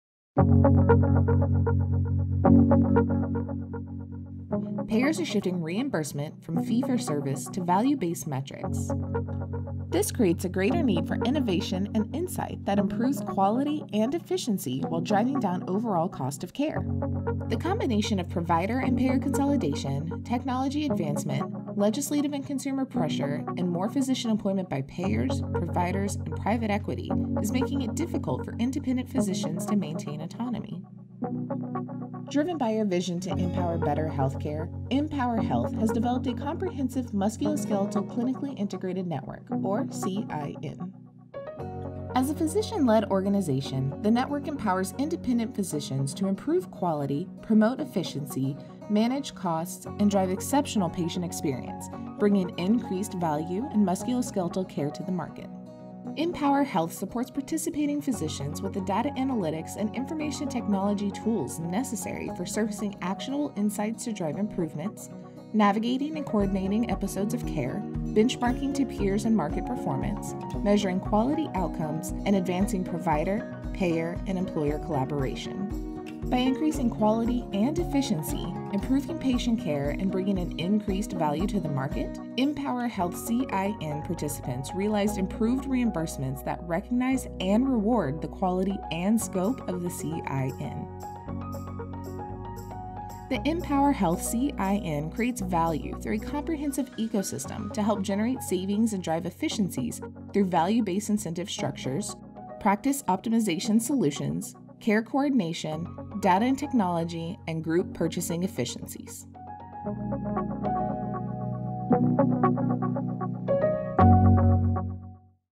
MPOWERHealth Clinically Integrated Network
An animated video of how the clinically integrated network works to help independent physicians survive and thrive in today's healthcare environment.